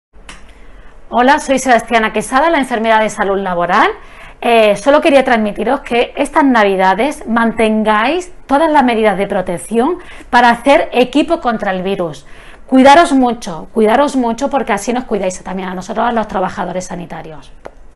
Aquest Nadal
La Sebas Quesada, Infermera de Salut Laboral del CLÍNIC, ens recorda que la situació epidemiològica actual obliga a extremar les mesures preventives

👉Reduir la interacció social
👉Reduir la mobilitat
👉Protegir els grups de risc
👉Mascareta
👉Higiene de mans
👉Distància
👉Ventilar espais

No t'arrisquis. CapFred.